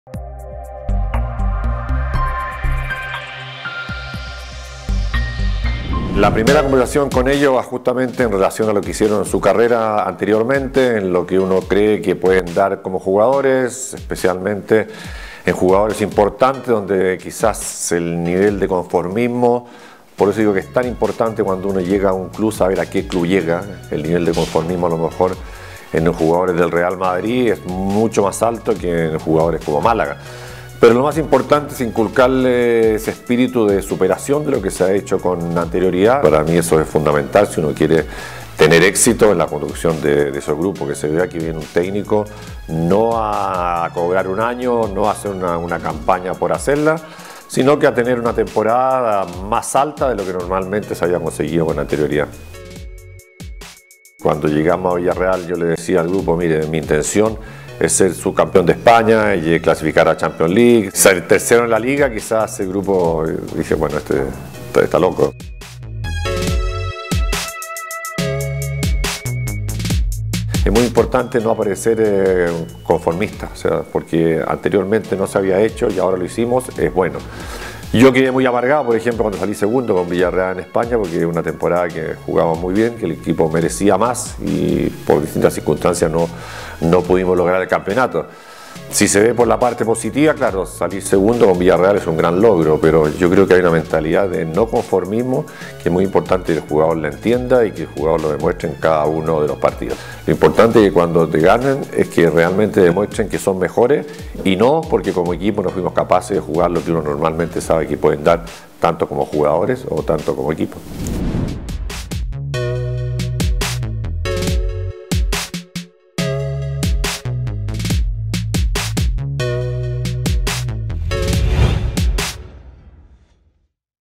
Charlas de Fútbol: Manuel Pellegrini y su manejo de plantel.
Manuel Pellegrini te cuenta como es su primera conversación cuando llega a un equipo de fútbol. El entrenador chileno llegó a dirigir clubes como Real Madrid, Manchester City, Villareal, Malaga, River Plate, Universidad Católica, Universidad de Chile, Palestino, Liga de Quito...